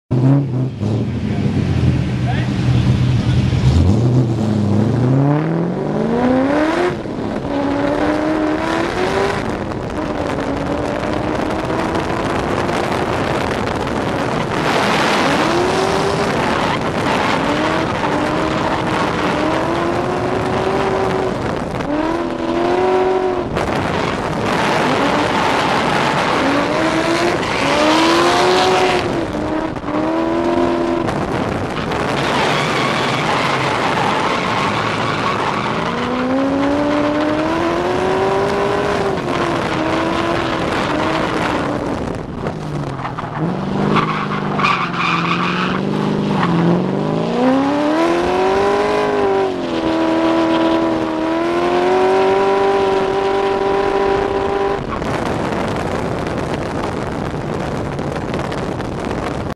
HotMoves xdc layout.
More footage from Clubloose's HotMoves 8/28/10 running the XDC layout.